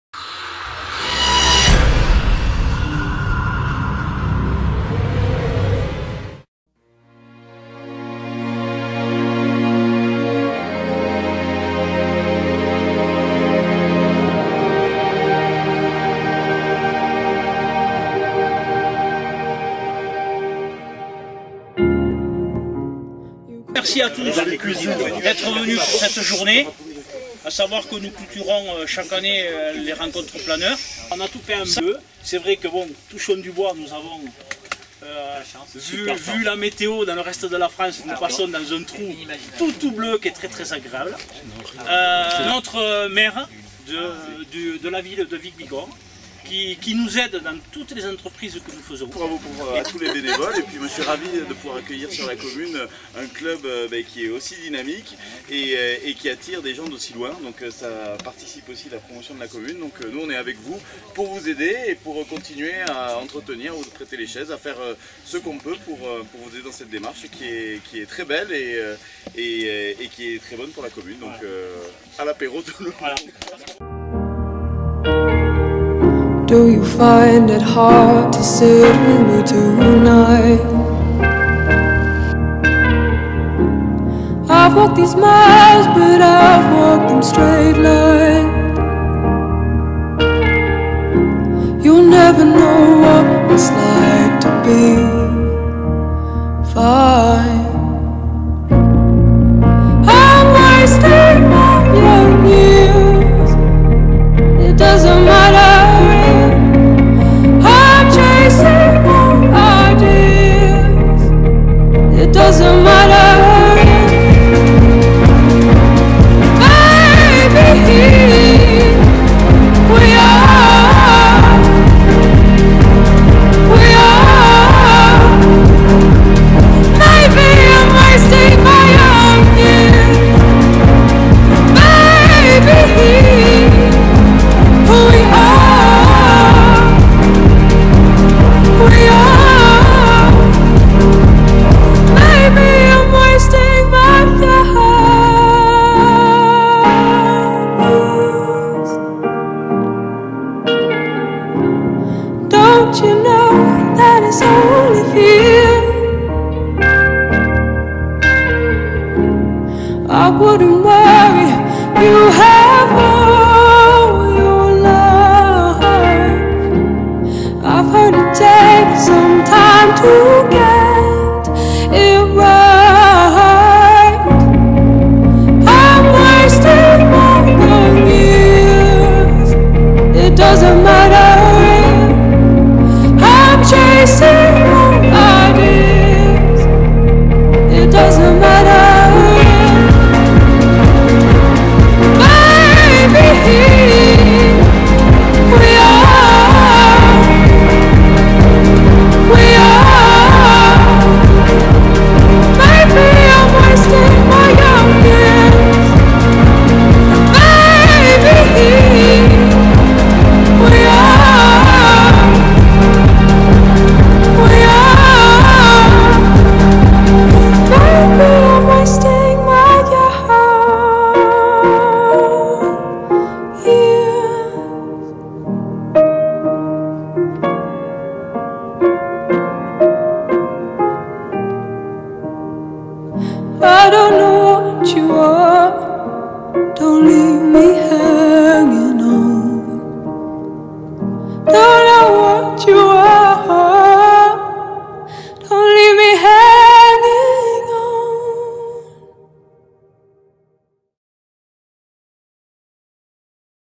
filml vic bigorre 2014
Rencontre GPR du 1er novembre 2014 à Vic en Bigorre qui a été supprimée suite à la fermeture d'un site dédié à l'aéromodélisme